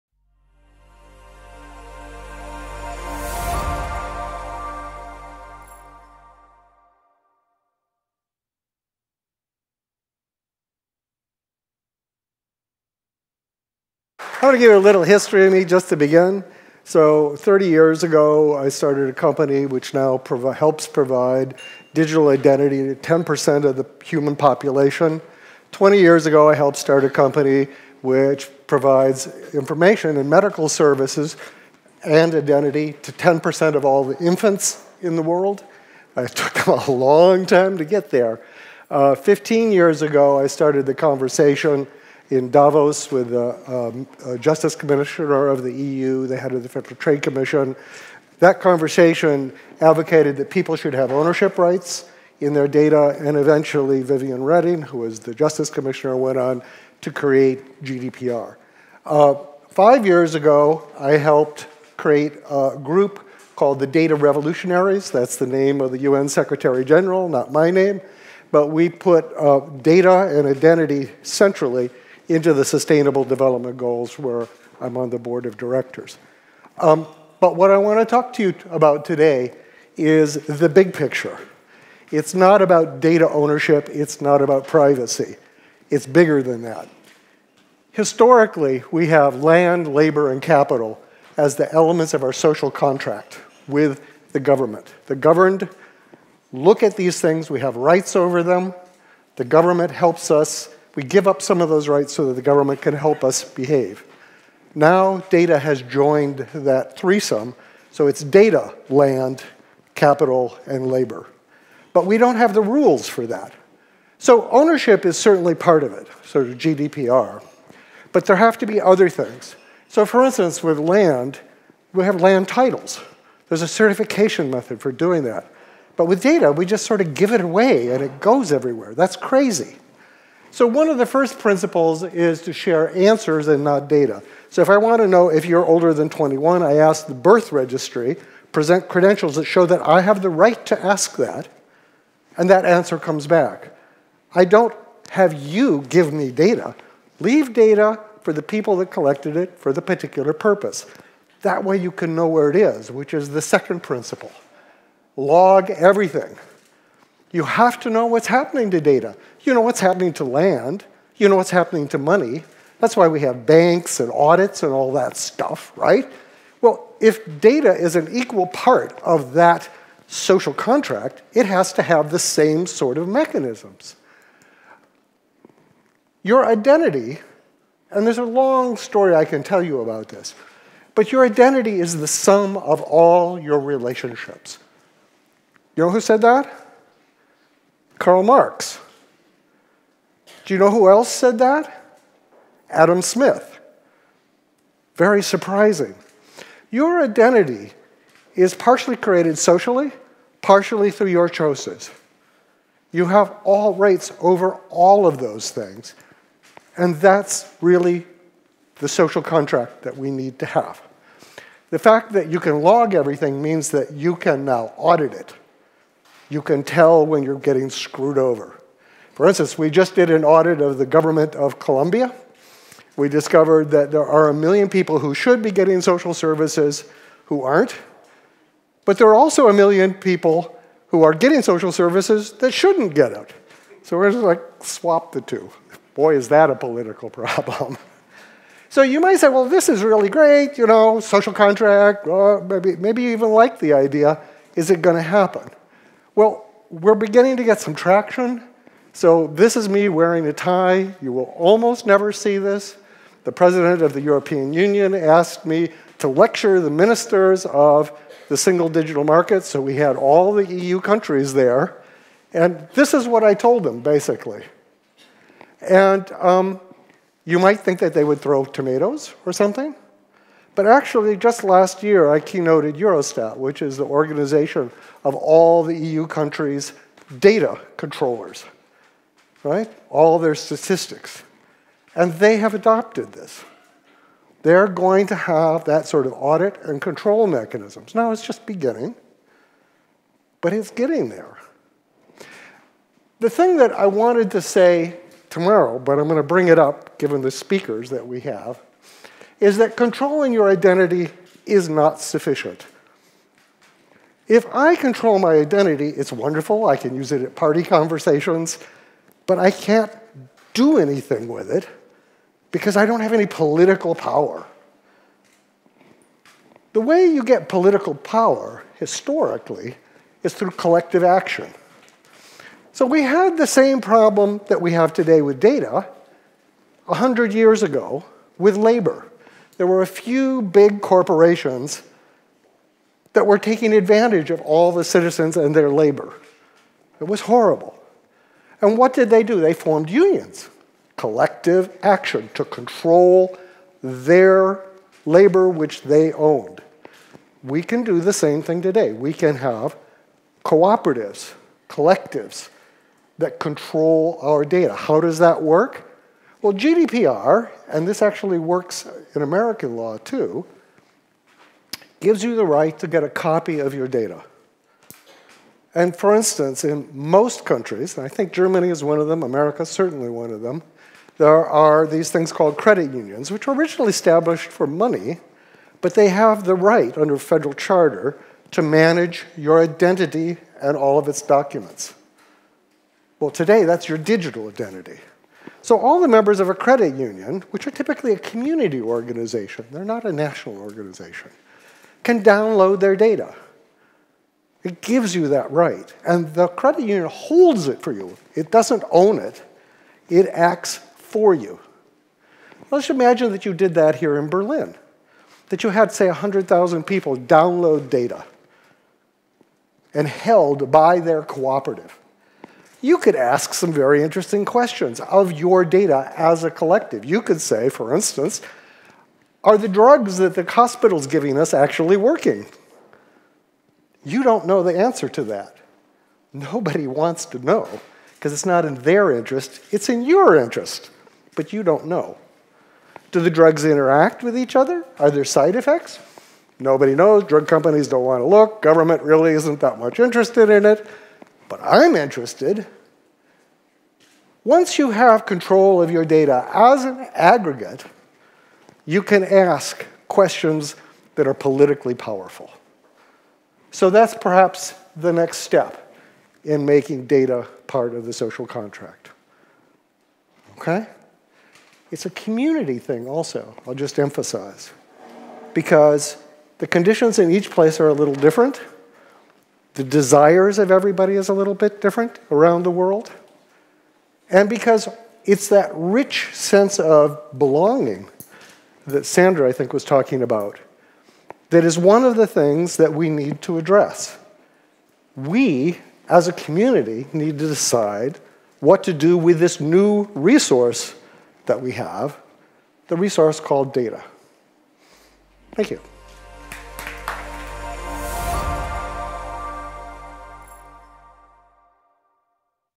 Sandy Pentland - The Future of Humans on Earth and Off
Sandy Pentland presents at the World Frontiers Forum Berlin, this fall 2019